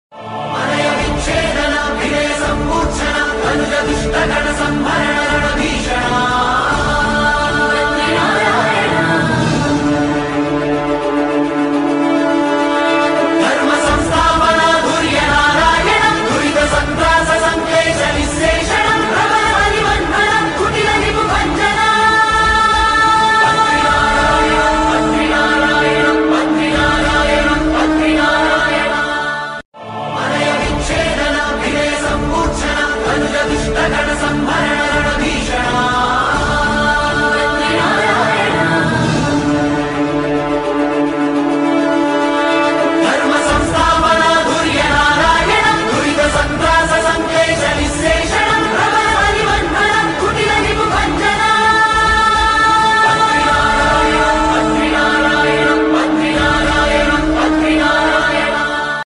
Easy trick to string flower garland / Nandivardhanam Garland/Mallipo Garland/Rosebay
Easy trick to string flower garland / Nandivardhanam Garland/Mallipo Garland @akondiharipriyadevotional